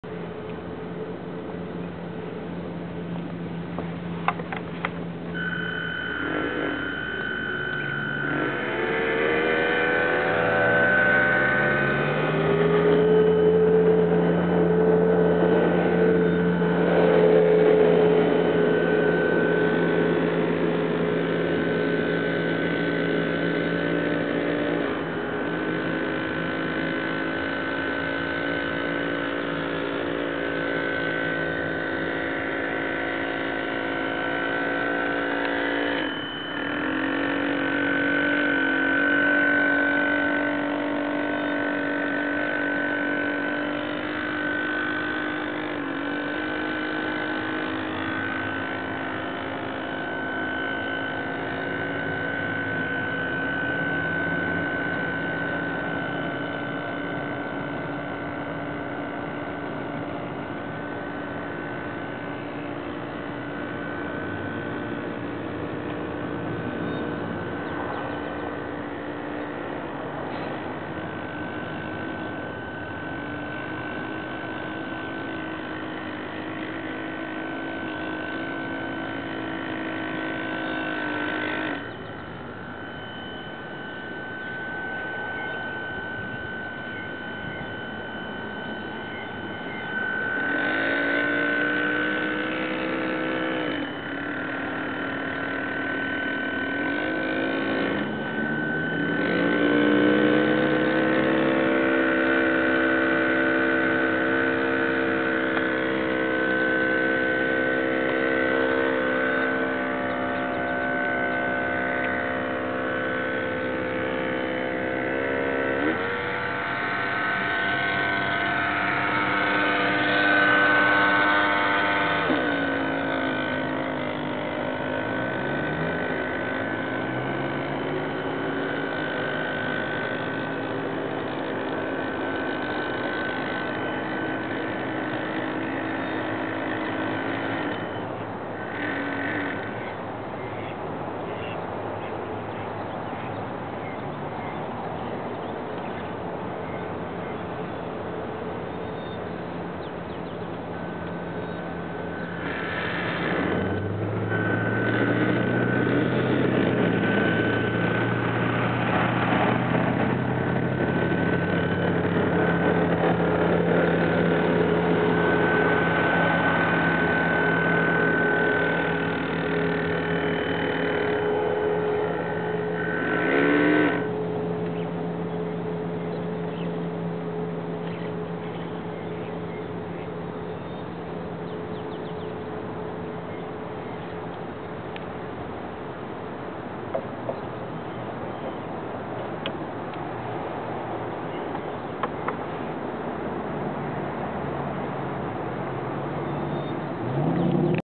Chris Craft 1955 Sterling 21' Monterey
Chris Craft 1955 Sterling 21'  Monterey, built in the mid 50's !
Spreckels Lake, SF, California.